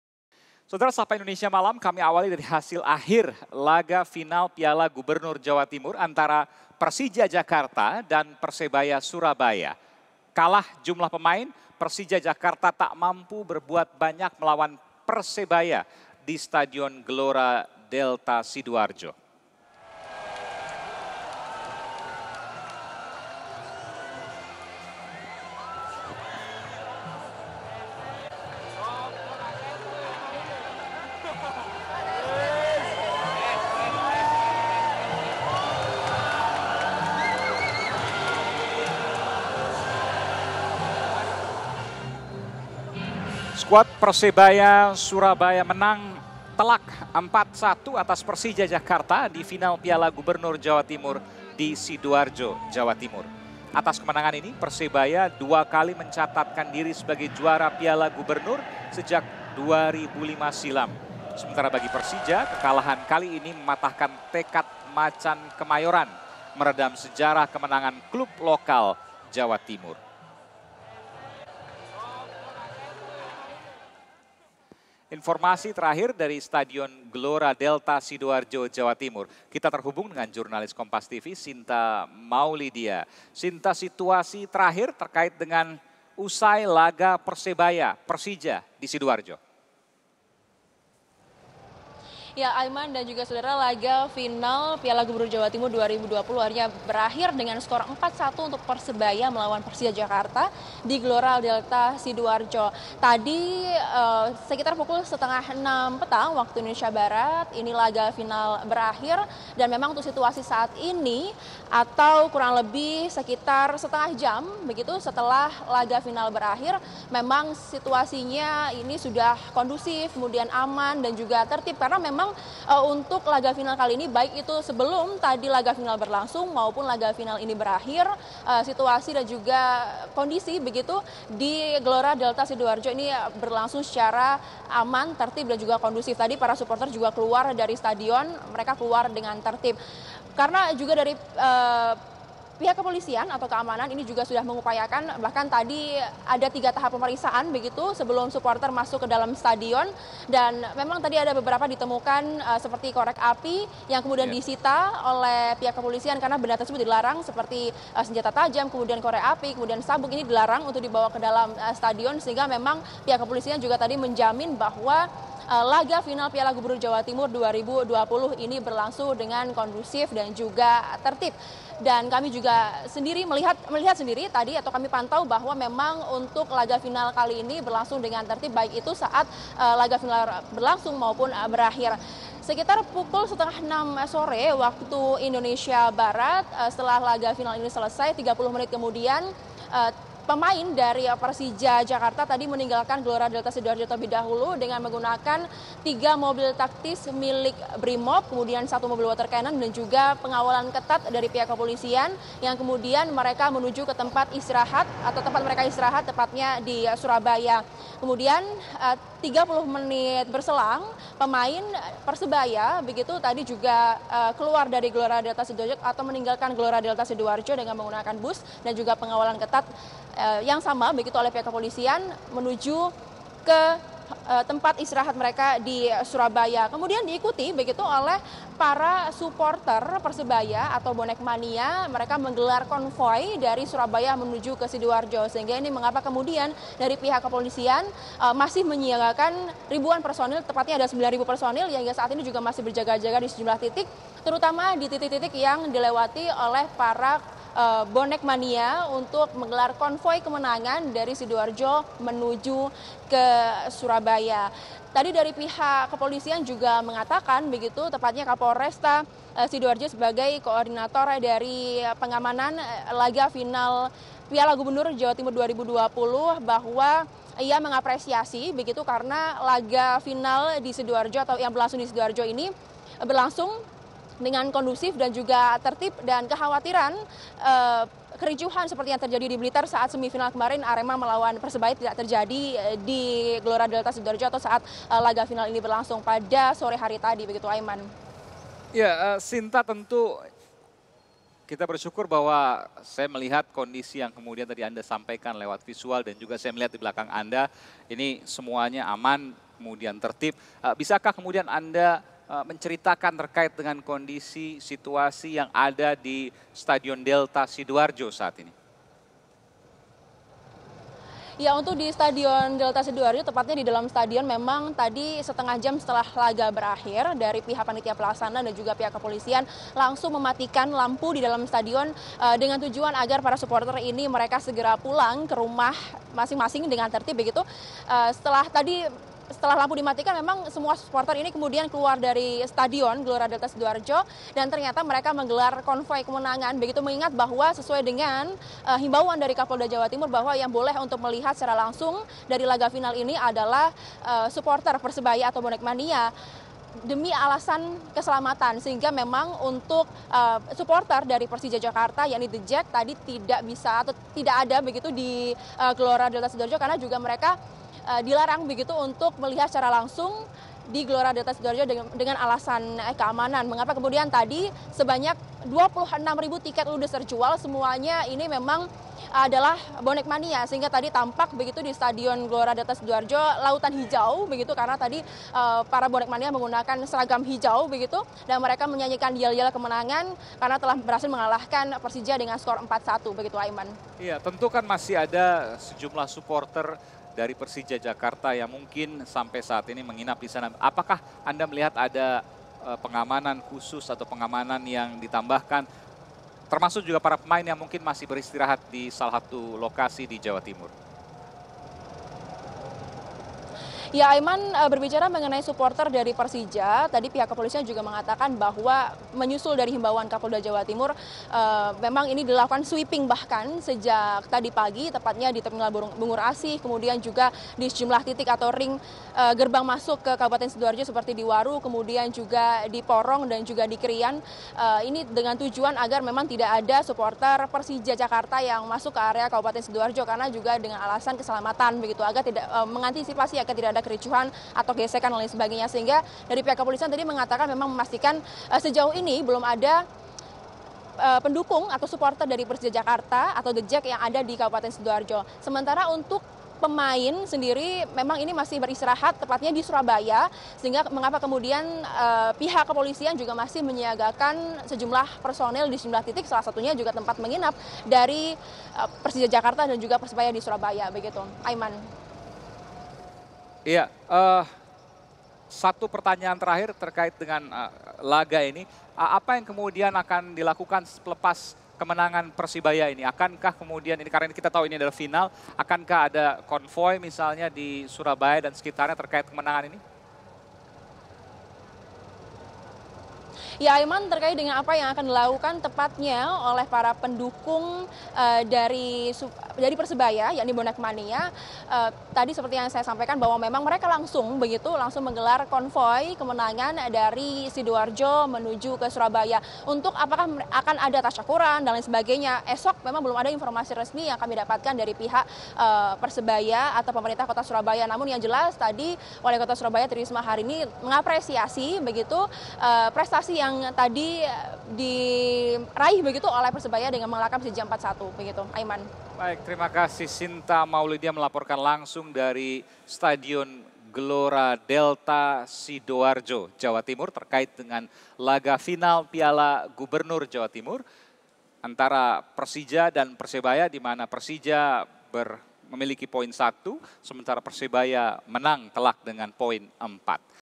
Jadi Juara! Persebaya Kalahkan Persija 4-1 di Piala Gubernur Jatim 2020
SIDOARJO, KOMPAS.TV - Hasil akhir laga Final Piala Gubernur Jawa Timur 2020, antara Persija Jakarta dan Persebaya Surabaya.

Kalah jumlah pemain, Persija Jakarta tak mampu berbuat banyak, melawan Persebaya di Stadion Gelora Delta Sidoarjo.

Squad Persebaya Surabaya, menang mutlak 4-1 atas Persija Jakarta di Final Piala Gubernur Jawa Timur, Sidoarjo, Jawa Timur.

Gol-gol Persebaya dicetak oleh empat pemain berbeda, yakni Oktafianus Fernando (3'), Makan Konate (52'), Ricky Kambuaya (56'), dan Mahmoud Eid (80').

Sementara itu, satu gol Persija diciptakan oleh tandukan Marko Simic lewat skema bola mati pada menit ke-44.

Beranjak menit-menit akhir, tensi permainan semakin panas.

Akan tetapi, tidak ada gol yang tercipta.

Laga berakhir dengan skor 4-1 untuk kemenangan Persebaya atas Persija Jakarta.

Atas kemenangan ini, Persebaya dua kali mencatatkan diri sebagai Juara Piala Gubernur, sejak 2006 silam.

Sementara bagi Persija, kekalahan kali ini lagi-lagi mematahkan tekad Macan Kemayoran, menghentikan dominasi kemenangan klub lokal Jawa Timur.